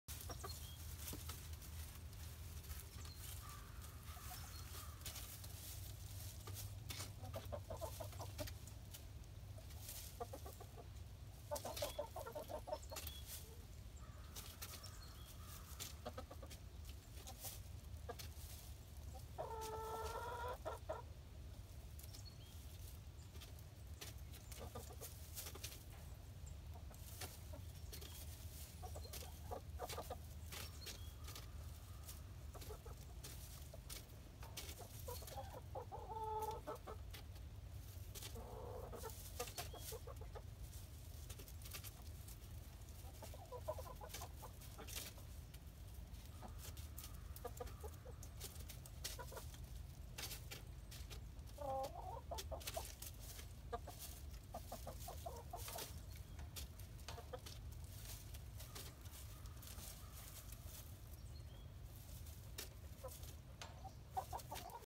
Chicken Tetherball
The Buff Orpingtons are winning, but everyone knows they cheat.